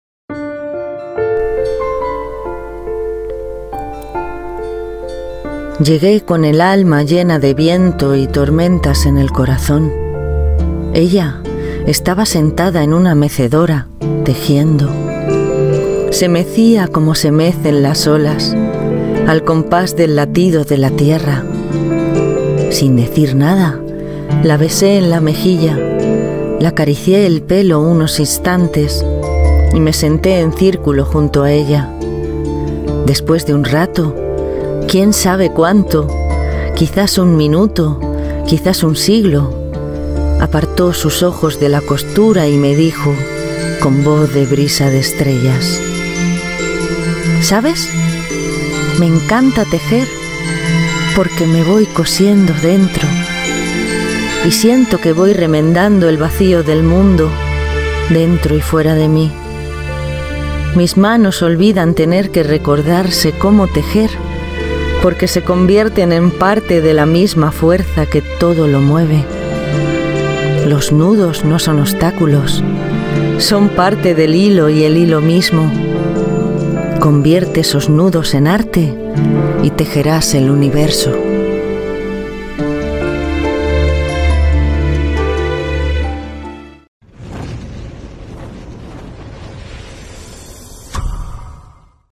La tejedora de mundos. Videopoema del Audiolibro "Brotes de esperanza". Ada Luz Márquez.
Videopoema "La tejedora de mundos". De mi primer Audiolibro "Brotes de esperanza"
Audiolibro "Brotes de esperanza", disponible en todas las plataformas digitales ( iTunes, AppleStore, AmazonMusic, Spotify...)